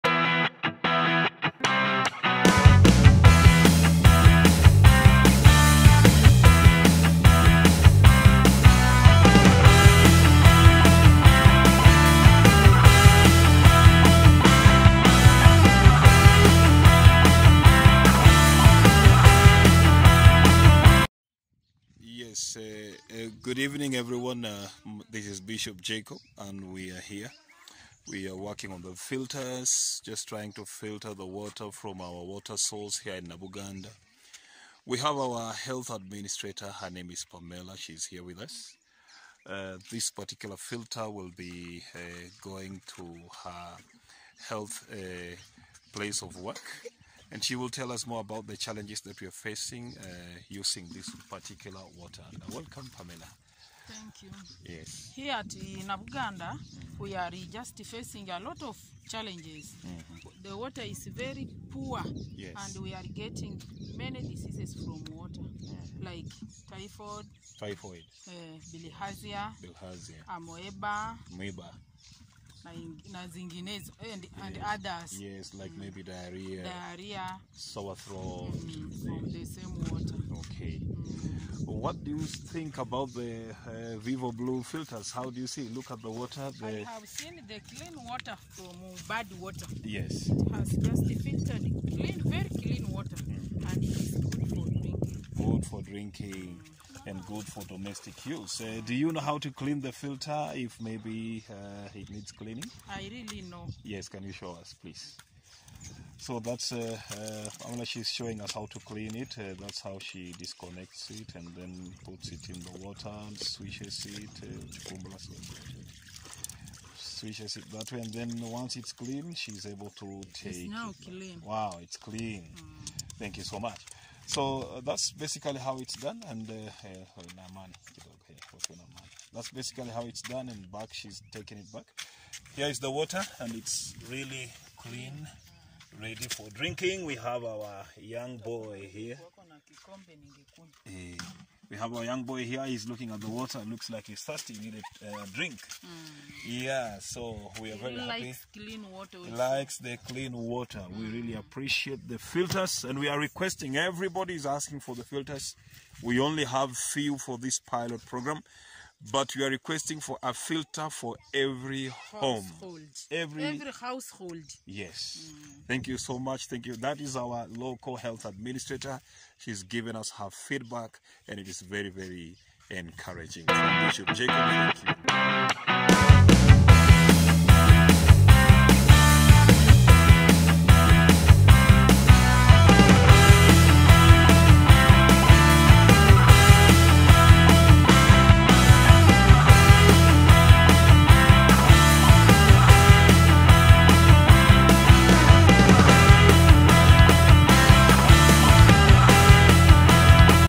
Global Water Works in Kenya with Bishop Jacob
Poor quality water results in the transmission of diseases such as cholera, diarrhoea, dysentery, hepatitis A, typhoid and polio.